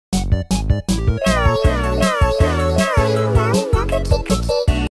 Small announcement (Read description)
I’ve decided that I might start posting Genshin impact/ cookie run kingdom content since I’ve been getting bored and unmotivated to post danganronpa content (I will still do danganronpa but it might take a little longer)